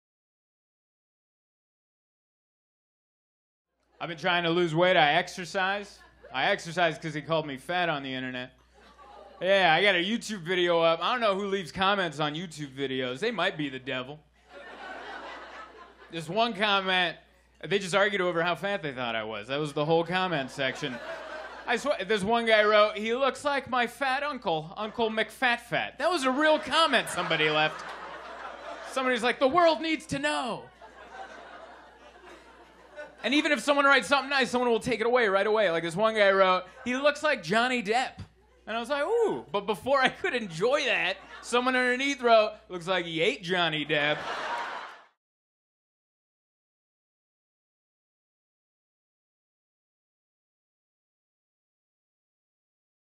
American Immigrant: Zoltan Kaszas - Preview 1
Short Preview of American Immigrant: Zoltan Kaszas, debut show at 2017 Edinburgh Fringe.